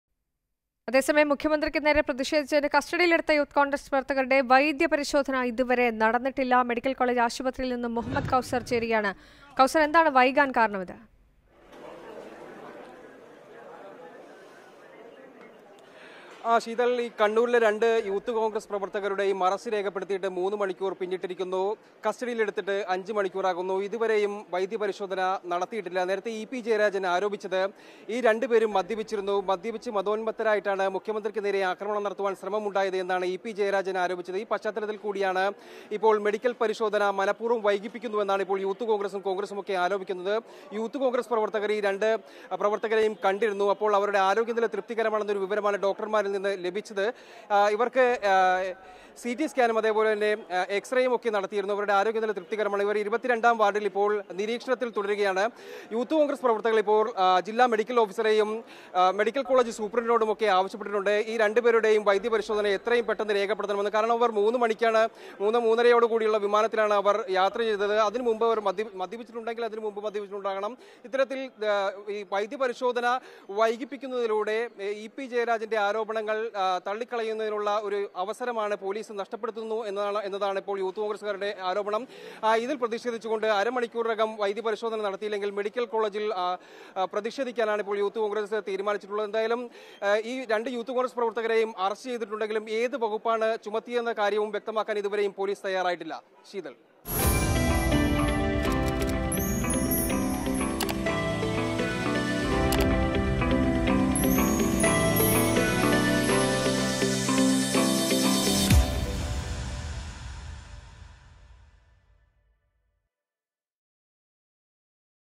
കസ്റ്റഡിയിലായവരുടെ വൈദ്യപരിശോധന വൈകുന്നു
മുഖ്യമന്ത്രിക്ക് നേരെ വിമാനത്തിൽ പ്രതിഷേധം നടത്തിയവരുടെ വൈദ്യപരിശോധന വൈകുന്നു, മനഃപൂർവ്വം വൈകിപ്പിക്കുന്നുവെന്ന് കോൺഗ്രസ്
protest against pinarayi vijayan | latest Updtaes